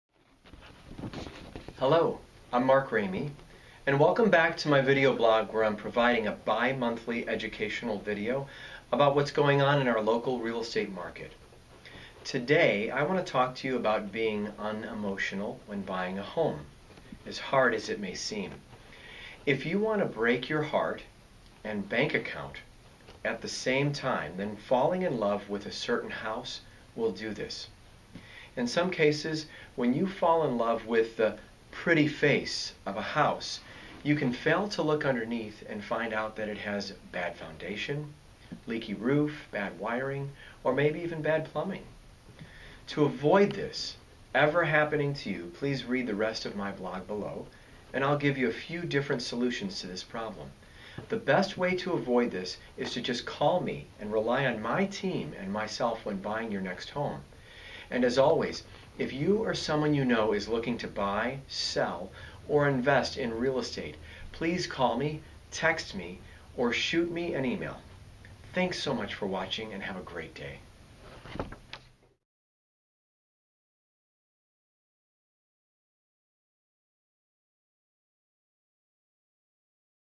Mark Ramey - Being Unemotional When Buying a Home
Want to break your heart and your bank account at the same time? Then buy a new home based on the fact you've fallen in love with it!  Needless to say, you should never do this! In some cases, when you fall in love with the "pretty face" of a house, you fail to look underneath and find problems like bad wiring, leaky roofs, bad foundations, etc. This is an extremely expensive way to buy a home! Visit our video blog and we'll teach you how to 'hide your emotions' when you shop for your next home!

Mark Ramey
The Ramey Group Keller Williams Real Estate
Orlando, FL